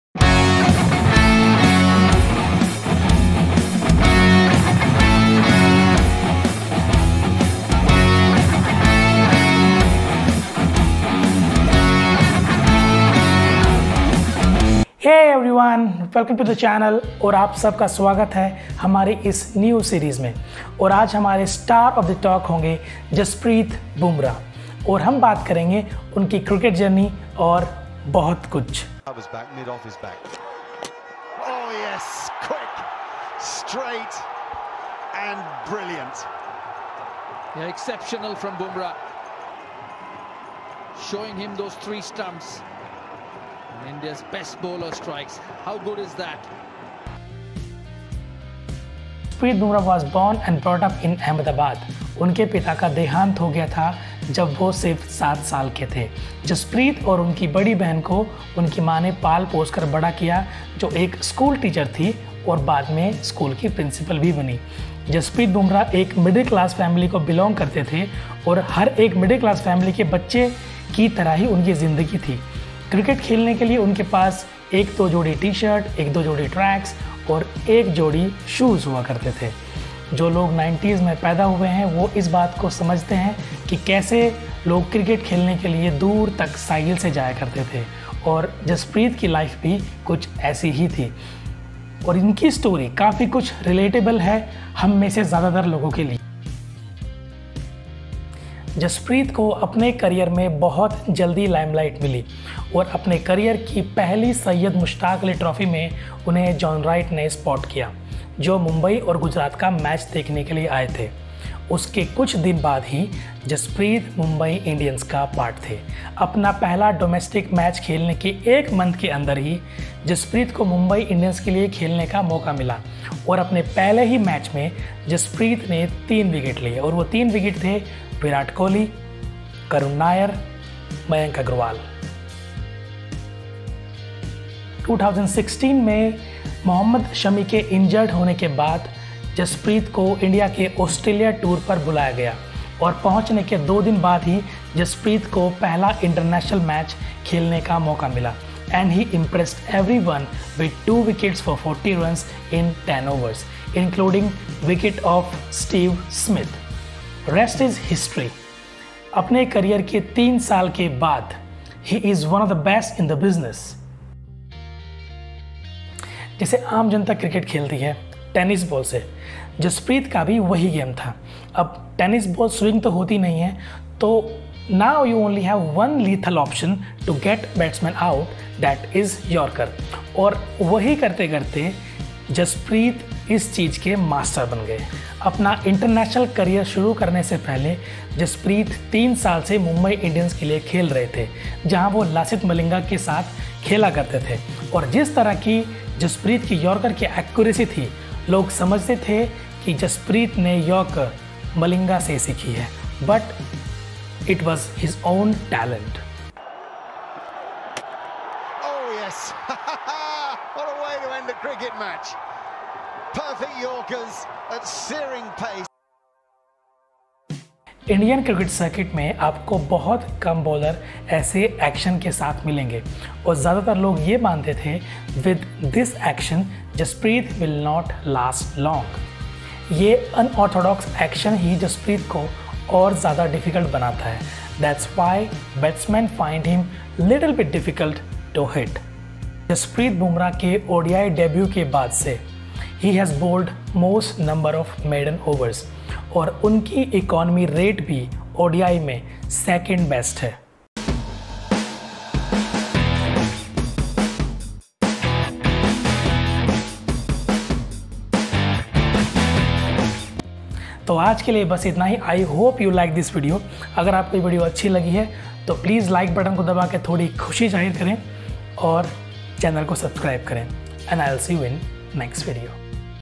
जसप्रीत बुमराह Biography | Boom Boom Bumrah | Story of Jasprit Bumrah
jasprit bumrah journey | jasprit bumrah | best yorker | bumrah bowling
In this Video, We talk about one of the generation talent Jasprit Bumrah's cricket journey and lot more.